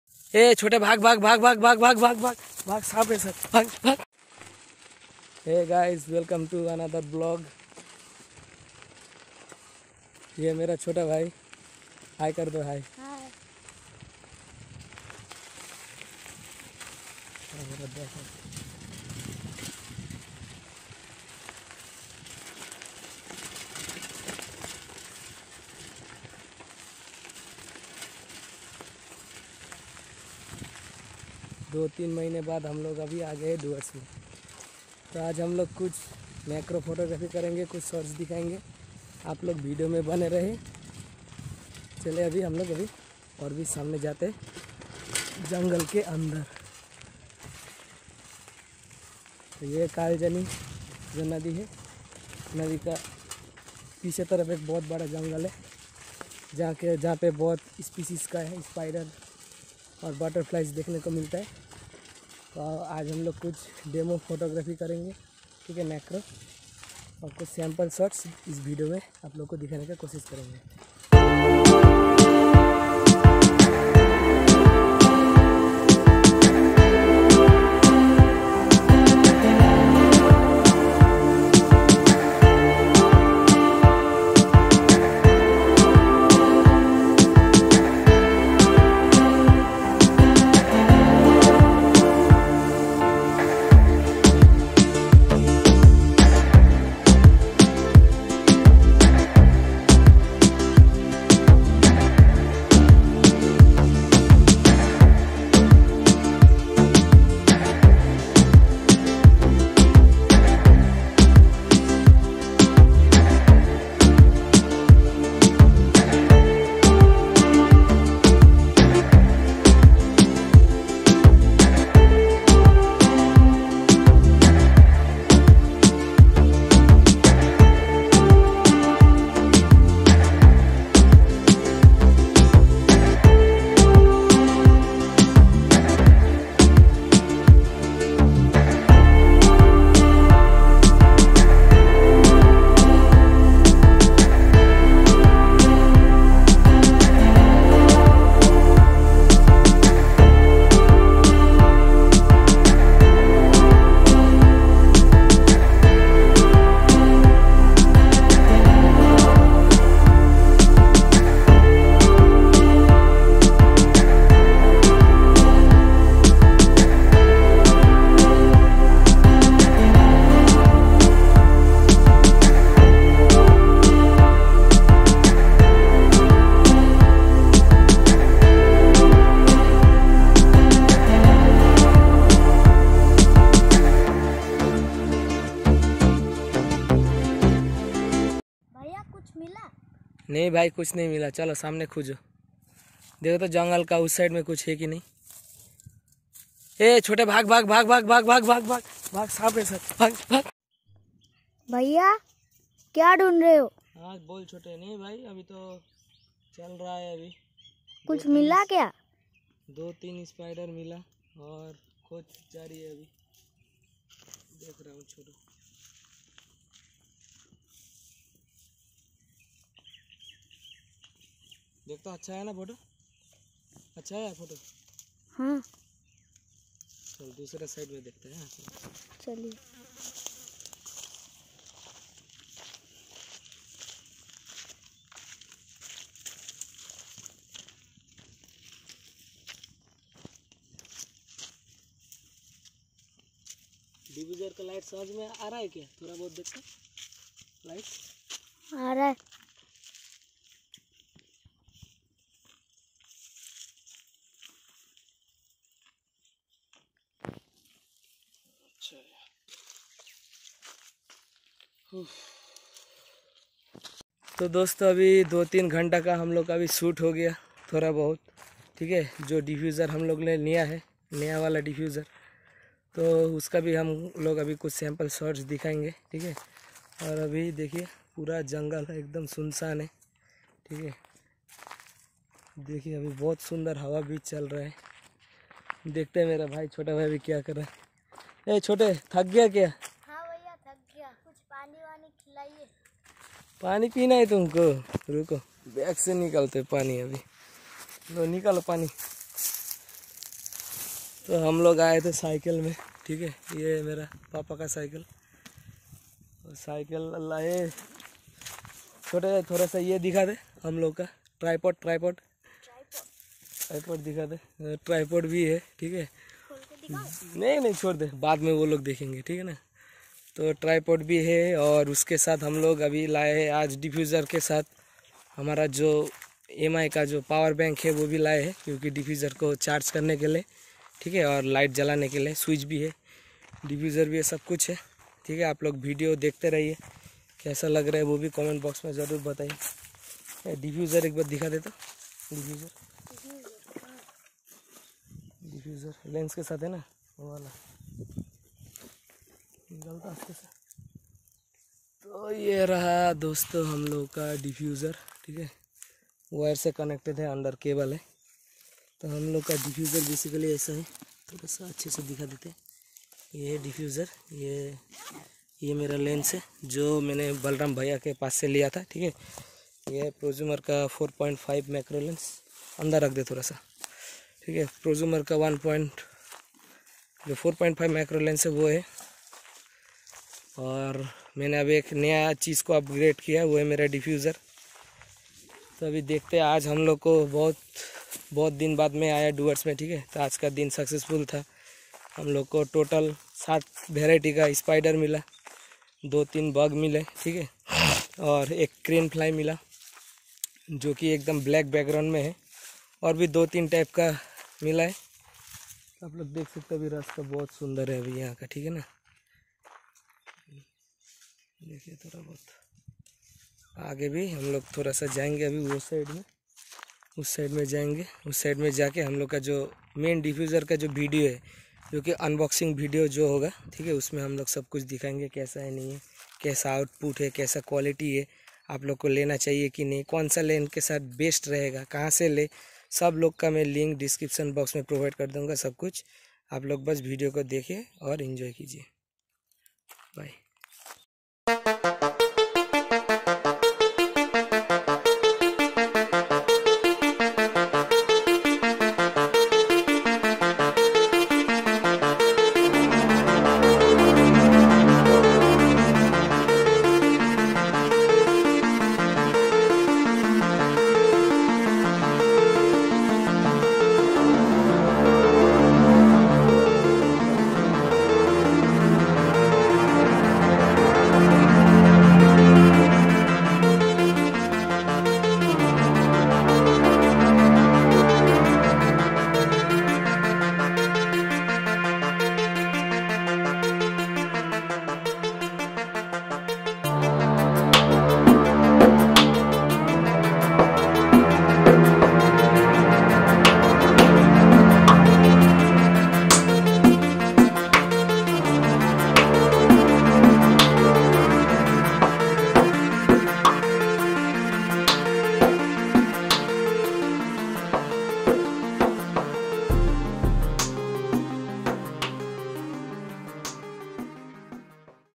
New Member Of Our Macro Family
New Member Added In Our Macro Family

1. Vivo V19 Mobile
2. Diffusers
3. Vivo Y33s Mobile Phone.
4. Tripod with holder.
5. Boya Mic.
6. Power Bank

🔶 My All Social Media Platform You Can Check Out 🔶

Please Do Follow And Support Me .